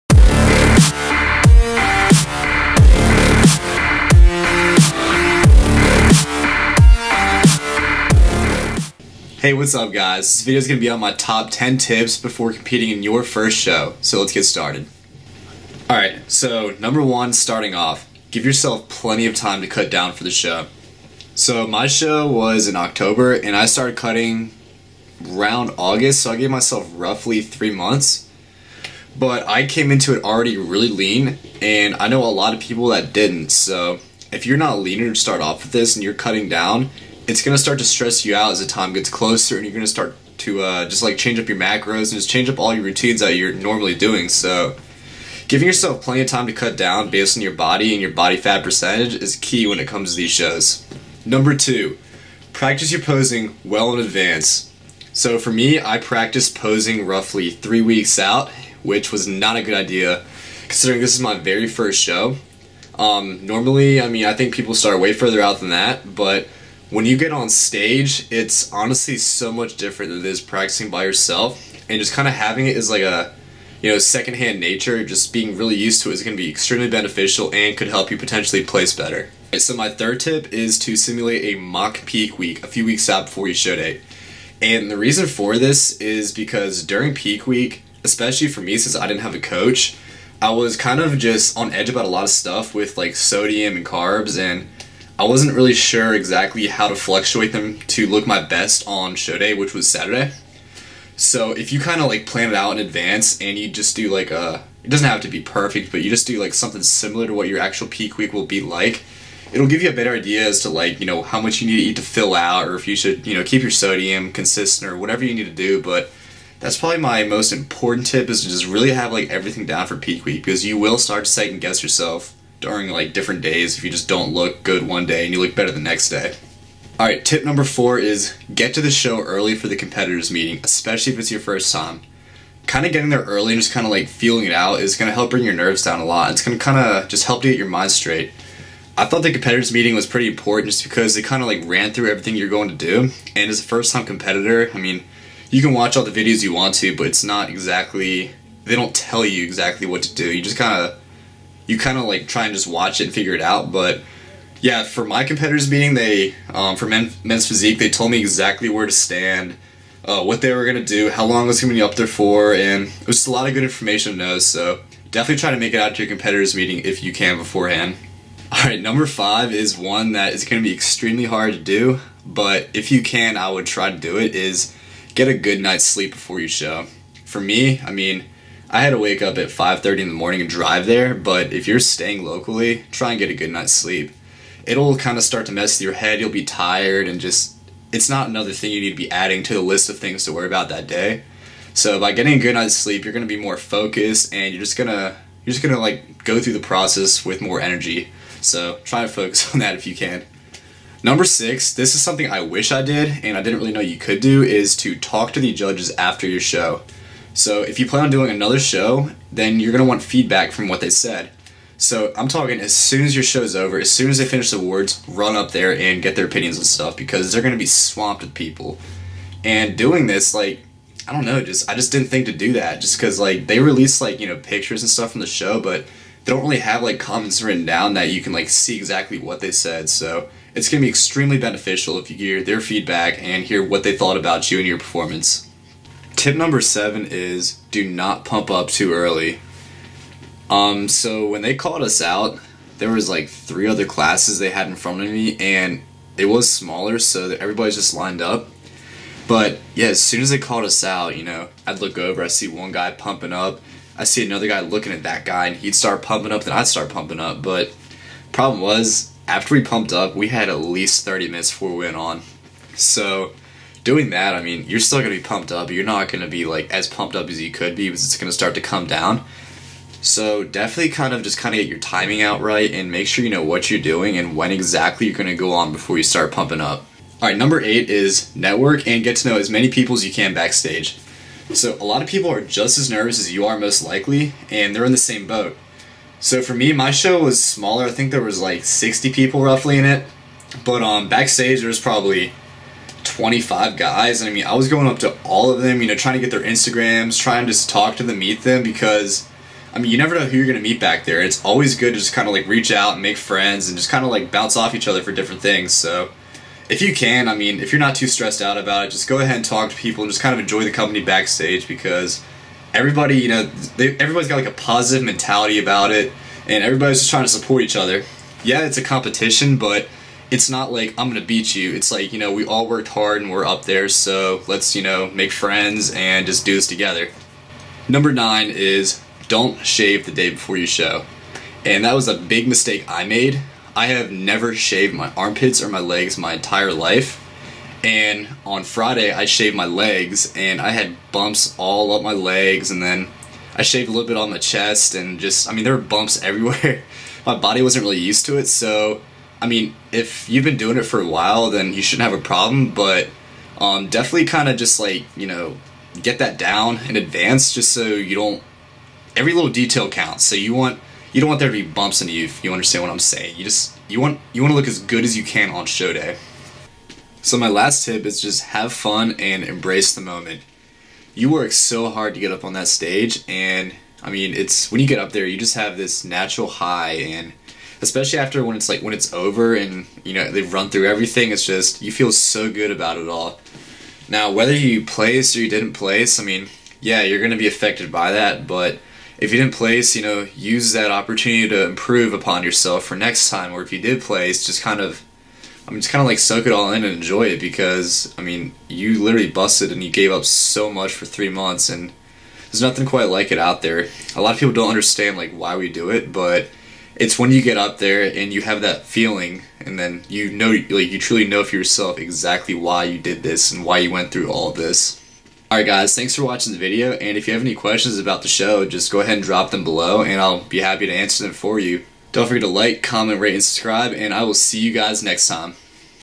My Top 10 Tips for Your First Fitness Competition!
Going into my competition, there were a lot of uncertainties with how to go about it. in this video, I talk about my top 10 things i learned or wish i had done better going into the show.